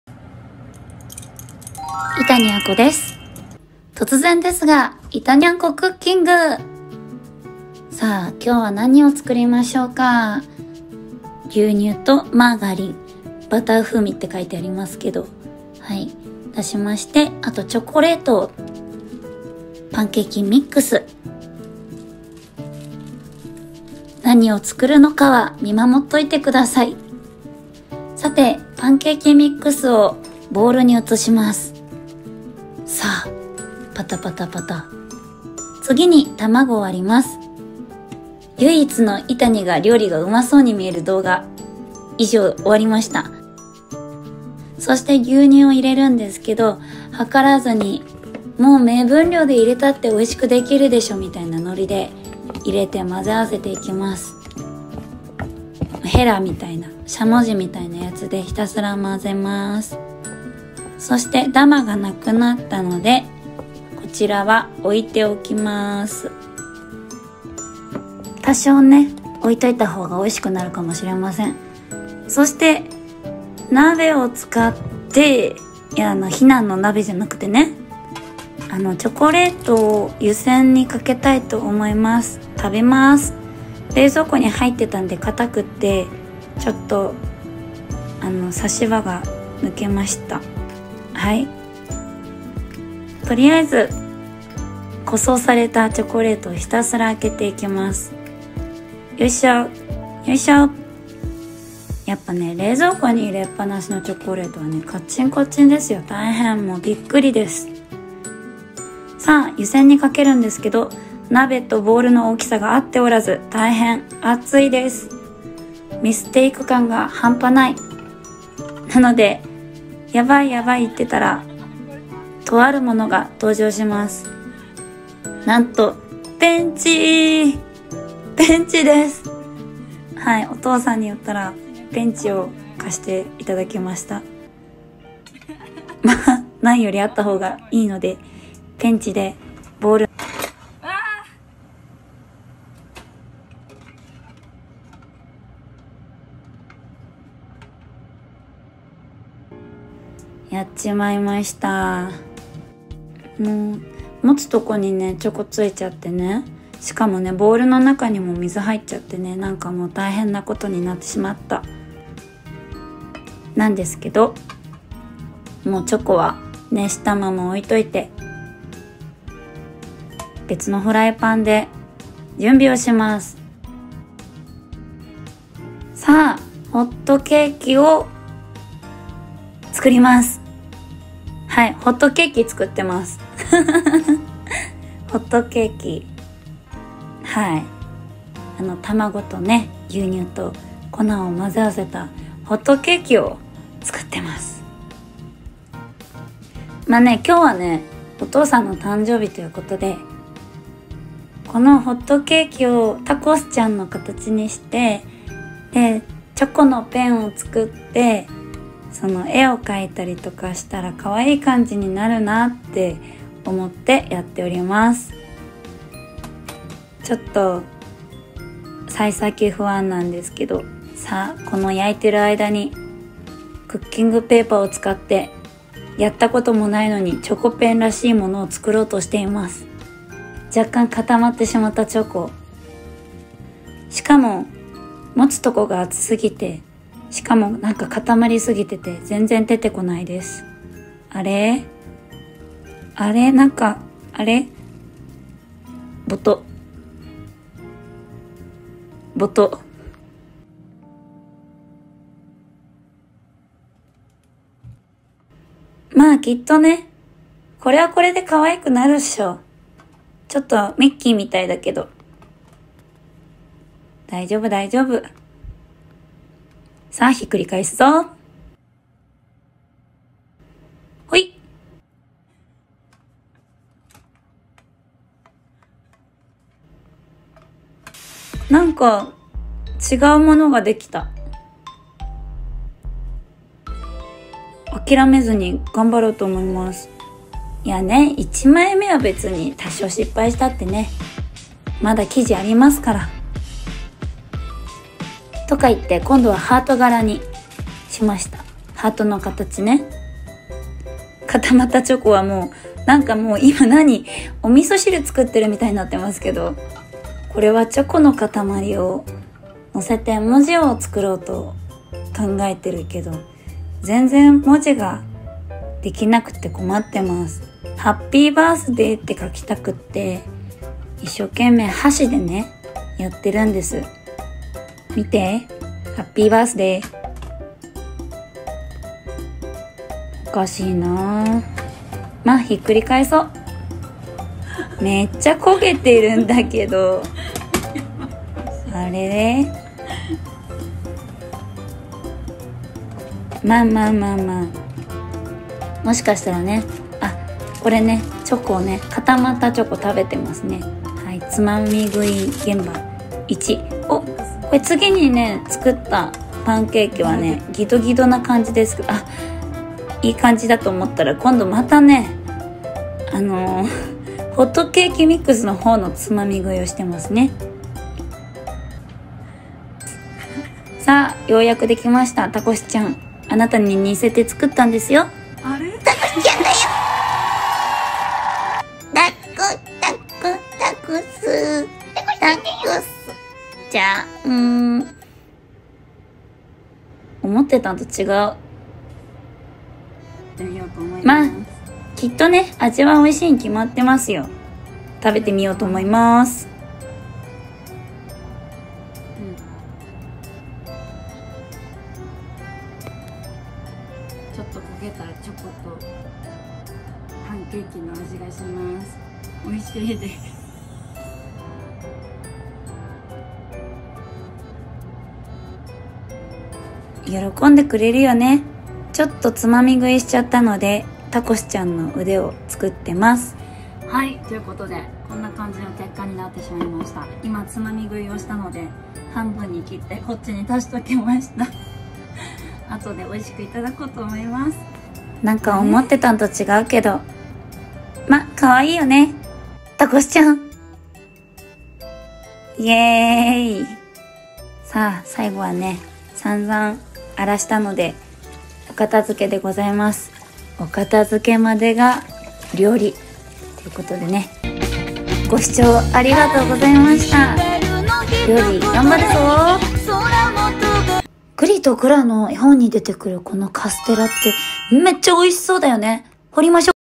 【シンガーソングライター×クッキング】父のお誕生日に料理を振る舞おうと思ったんだけど...
わたしが料理が得意ではなさそうなだけであって、
ほかのシンガーソングライターさんとは関係ありません。笑